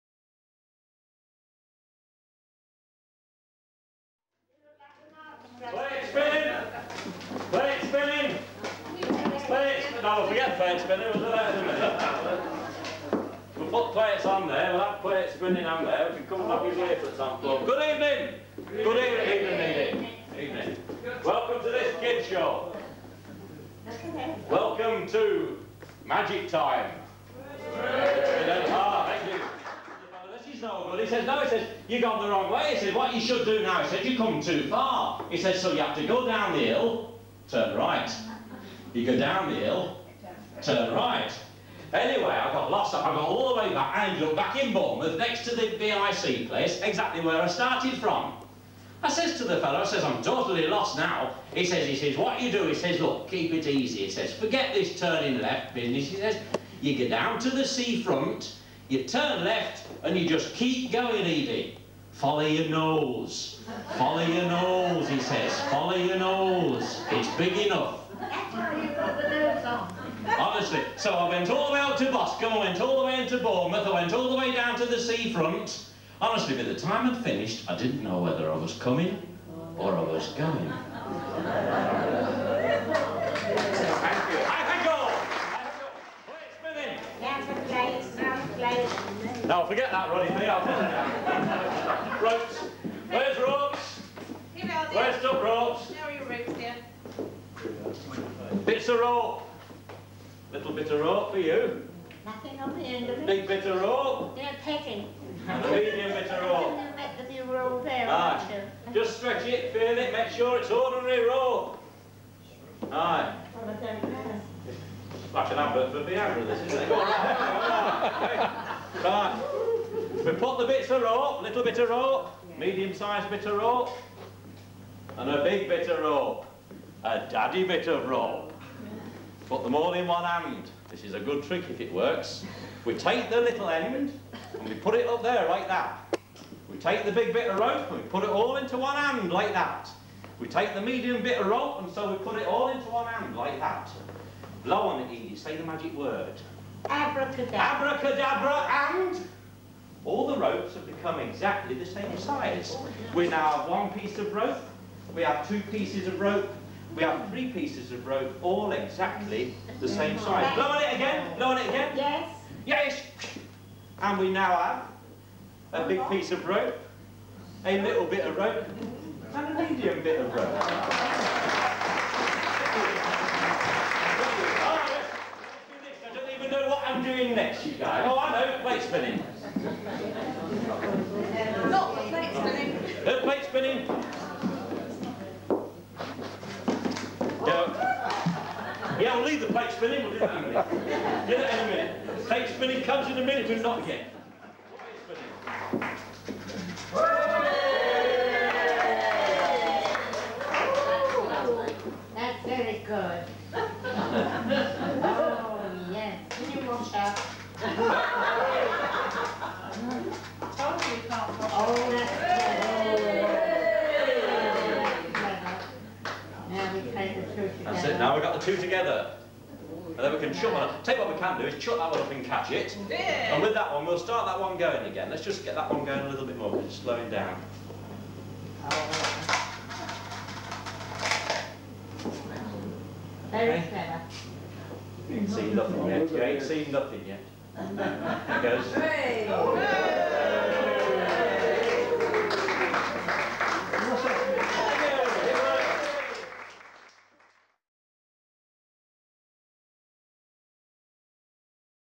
Magic Time - Penny & Peter.WMV
Just to give the audience (at a small, friendly organ festival) a bit of variety, and a break from the music, Peter Hayward and Penny Weedon put down their keyboards and presented a little magic show... just for fun. Here are a few clips from that show, including the plate-spinning... and some brilliant comments from a lovely lady (and great character) - Eadie, our No.1 fan !!